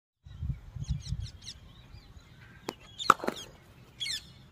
Cut shot
welcome to cricket with Sachin bora batting this is cut shot again leg spin ball